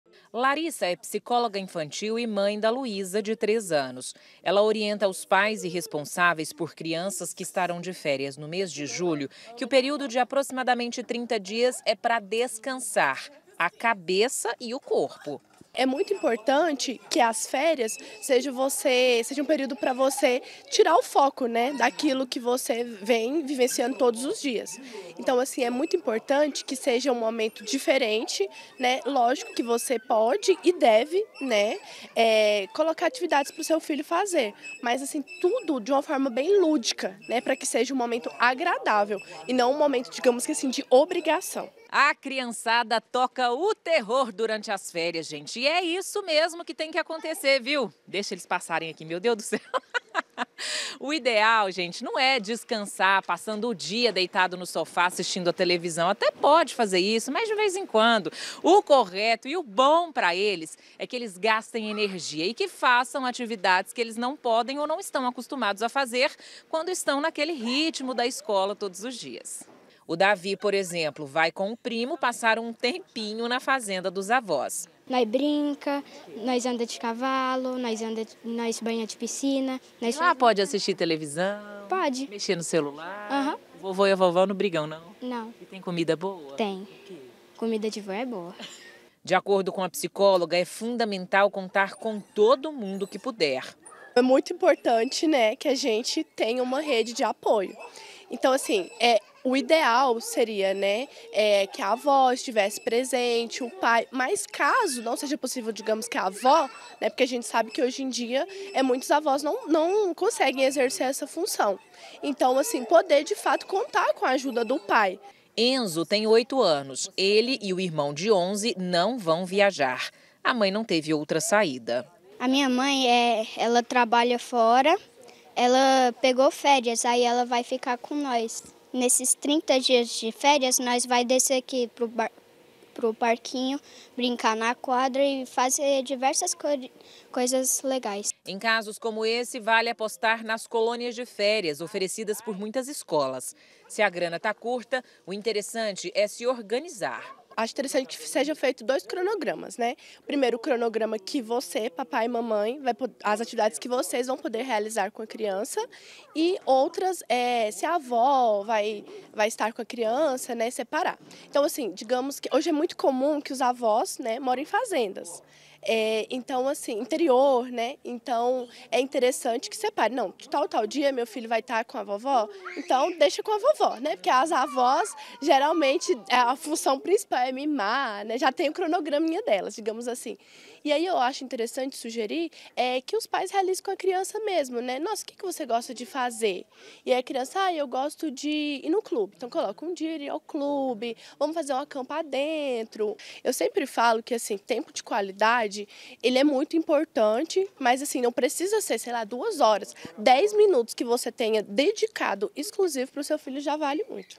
CRIANÇAS EM CASA: REINVENTAR AS FÉRIAS DE JULHO É A SOLUÇÃO
JULHO É MÊS DE FÉRIAS ESCOLARES, SE NÃO DÁ PRA VIAJAR, O JEITO É USAR A CRIATIVIDADE PRA ELAS NÃO FICAREM FRUSTRADAS.